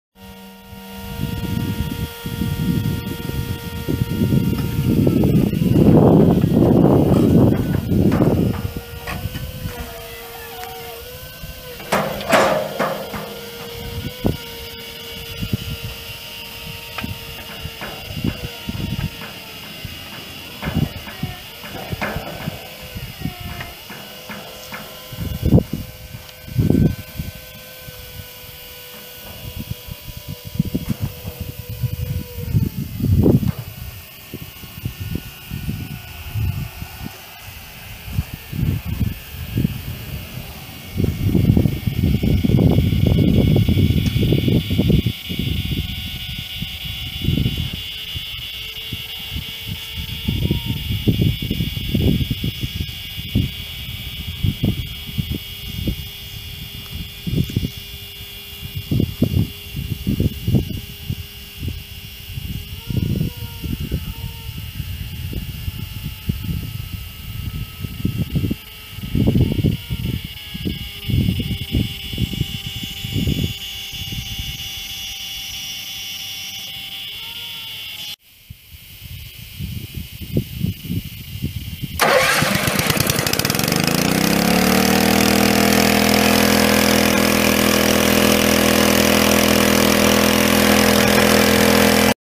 1998 Niftylift TM34HGE boom lift for sale at auction | bidding closes October 25, 2018
1998 Niftylift TM34HGE boom lift
Honda single cylinder gas engine
5.5 HP
24 volt electric motor
500 lbs maximum lift capacity
34' maximum lift height
24" x 36" platform
Platform and ground controls
Hydraulic outriggers
2" ball hitch
Jack stand
Torsion axle
175/80R13 tires
Non-operational hour meter
Non-operational safety switches
Serial 01-5803
01-5803
It's located in Breese, IL, and we're selling it to the highest bidder regardless of price.